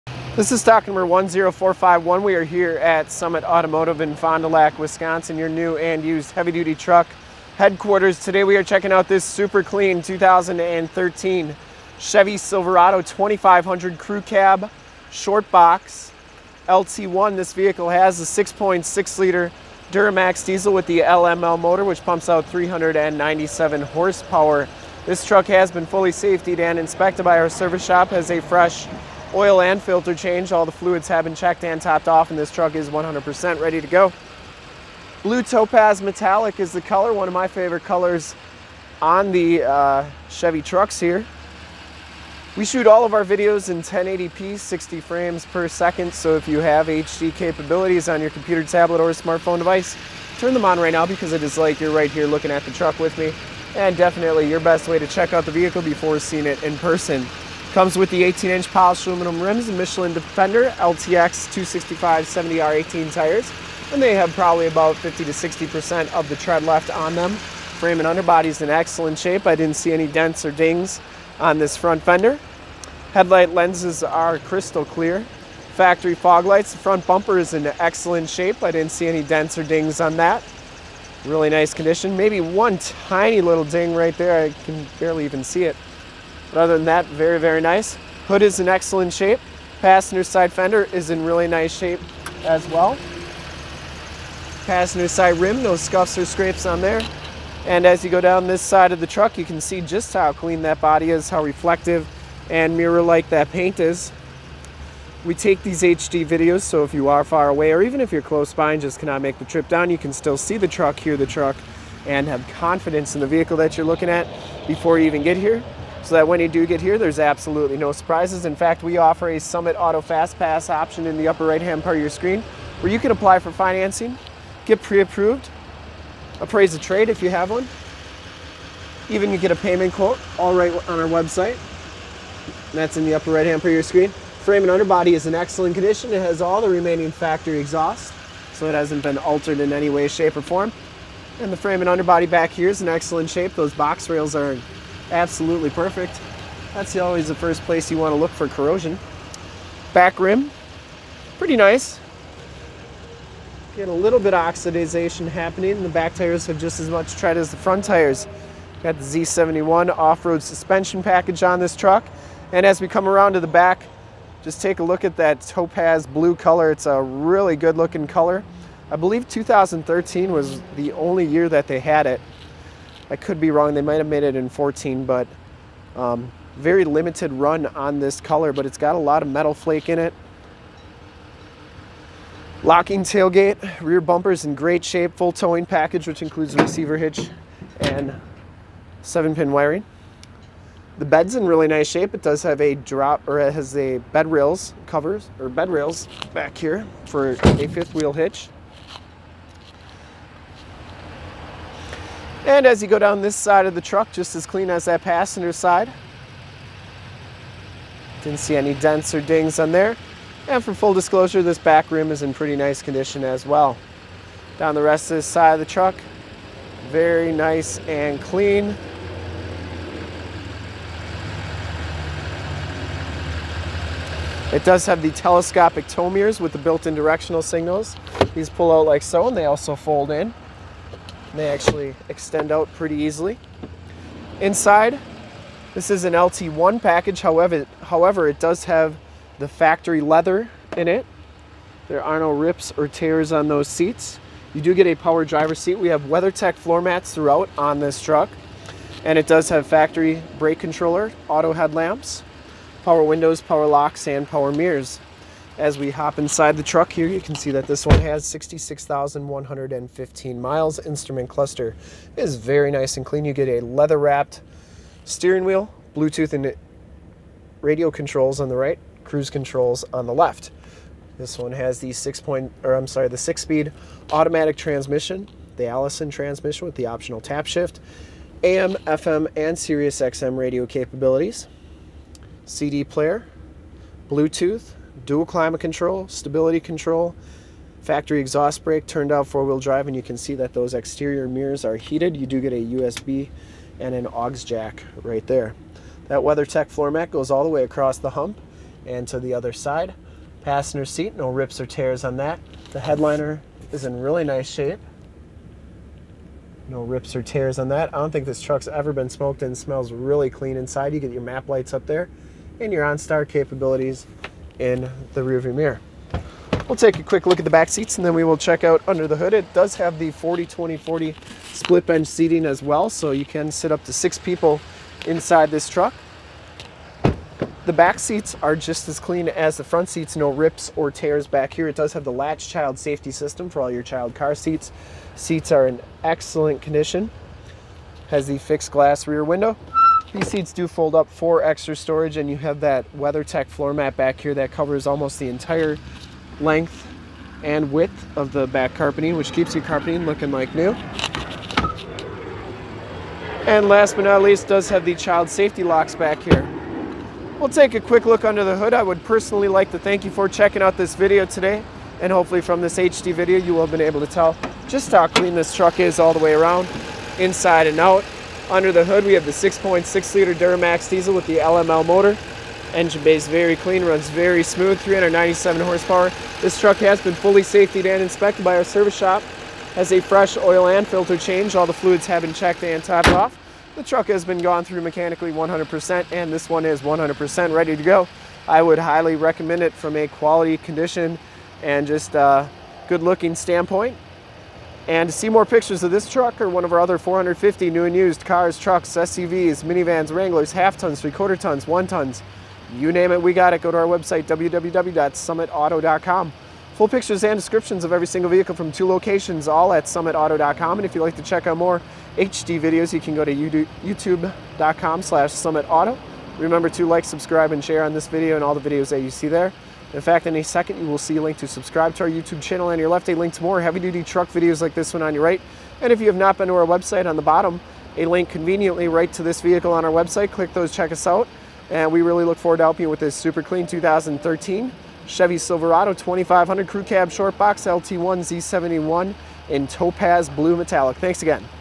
2013 CHEVROLET SILVERADO 2500 DURAMAX DIESEL TOPAZ BLUE WALK AROUND REVIEW SUMMITAUTO 10451 SOLD!
This 2013 CHEVROLET SILVERADO 2500 CREW DURAMAX DIESEL IN TOPAZ BLUE METALLIC is the vehicle we did a walk around review of today

Thank you for checking out this video of this 2013 CHEVROLET SILVERADO 2500 CREW DURAMAX DIESEL IN TOPAZ BLUE METALLIC

STOCK: 10451
PRICE: $35,999
MILES: 66,104
MAKE: CHEVROLET
MODEL: SILVERADO 2500
VIN: 1GC1KXC8XDF142464
LOCATION: FOND DU LAC OSHKOSH WISCONSIN, 54937 TRUCKS ON 41

1 OWNER! CLEAN TITLE HISTORY! 6.6 Liter V8 Duramax Diesel, LML Engine with 397 Horsepower, Full Four Door Crew Cab, Short Box 6 1/2 Foot Shortbox, LT1 Package 1LT LT, 6 Speed Automatic Allison Transmission with Optional Manual Tap Shift, Turn Dial 4x4 Four Wheel Drive 4WD, Z71 Offroad Suspension Package Z-71, Onstar System, Power Driver's Seat, Non Smoker, Black Ebony Leather Seats, 40/20/40 Split Front Bench Seating with Center Seat Hidden Storage Compartment, Full Towing Package with Receiver Trailer Hitch, Wiring and Transmission Cooler Tow Package, Factory Brake Controller, Fifth Wheel Bedrails 5th Wheel , Factory Exhaust Brake, Heated Telescopic Tow Power Mirrors with Built-in Directional Signals, Stabilitrak Traction Control, 3.73 Gears with Automatic Locking Differential Limited Slip Differential, Polished Aluminum Rims Premium Wheels, Four Wheel Disc Brakes, Aftermarket Chromed Stepbars, Fog Lights, EZ Raise Assist Tailgate, Locking Tailgate, AM / FM Radio Tuner, Sirius/XM Satellite Radio Capabilities Sirius / XM, CD Player, Bluetooth, Hands-Free Phone Controls Blue Tooth, Auxiliary MP3 Jack Portable Audio Connection, USB Jack Portable Audio Connection, Keyless Entry System, Adjustable Height Seatbelts, Driver and Passenger Front Air Bags, L.A.T.C.H. Child Safety System, Multi-Function Steering Wheel Controls, Dual Multi-Zone Climate Control , Weathertech Floormats, Air Conditioning AC, Cruise Control, Power Locks, Power Windows, Tilt Steering Wheel, Automatic Headlights Autolamp, Blue Topaz Metallic, ONE OWNER! CLEAN AUTOCHECK! Very very clean inside and out! This is one of the sharpest 2013 Chevrolet Silverado 2500 crewcab shortbox 3/4 ton diesel trucks on our lot! Make your move before this super clean 4wd is gone!

STOCK: 10451
PRICE: $35,999
MILES: 66,104
MAKE: CHEVROLET
MODEL: SILVERADO 2500
VIN: 1GC1KXC8XDF142464